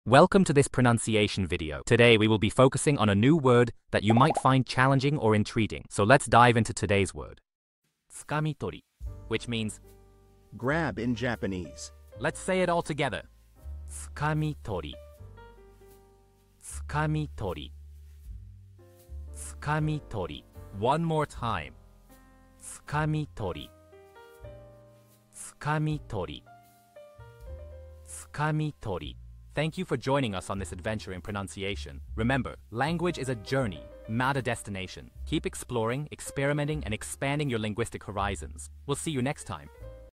How to pronounce tsukamishu | つかみ取 (Grab in Japanese)
Master the Pronunciation of 'tsukamishu | つかみ取' - which means : Grab in Japanese 📝 with @PronunciationJapanese 🎓🗣️ - [Your Guide to Japanese]

Learn to pronounce 'tsukamishu' perfectly in Japanese with our easy and engaging tutorial by @PronunciationJapanese! 📖📽️ In this comprehensive video, we provide you with native speaker examples 🗣️👂 and phonetic breakdowns 🎵 to ensure you get the pronunciation just right.